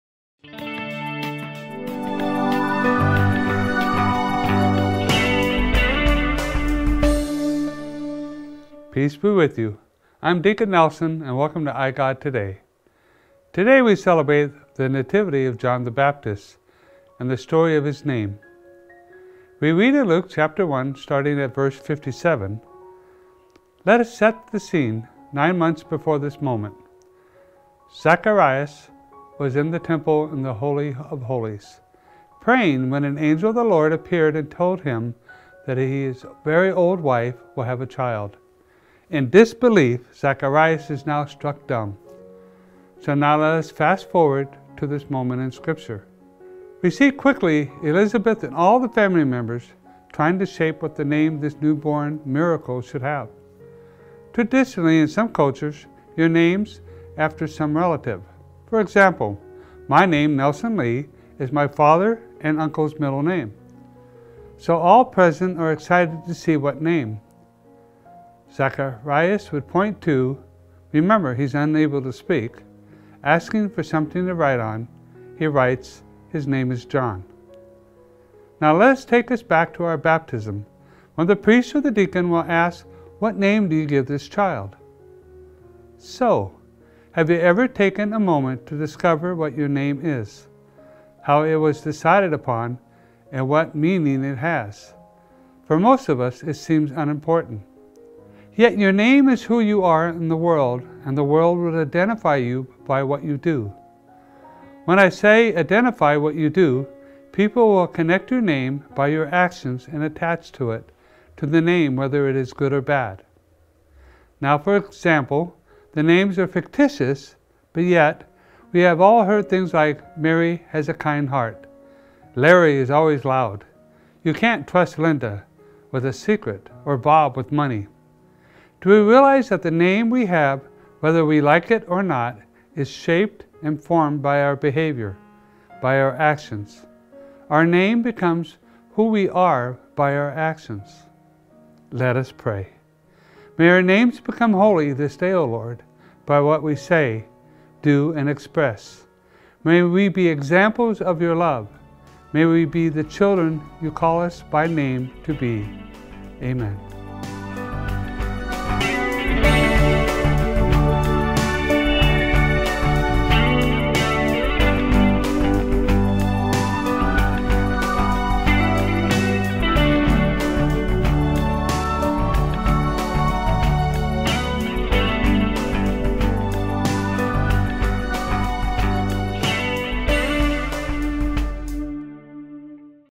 iGod Today with Dn. Nelson Glass; Today's topic: Nativity of Saint John the Baptist
When the time arrived for Elizabeth to have her child she gave birth to a son. Her neighbors and relatives heard that the Lord had shown his great mercy toward her, and they rejoiced with her. When they came on the eighth day to circumcise the child, they were going to call him Zechariah after his father, but his mother said in reply, “No. He will be called John.”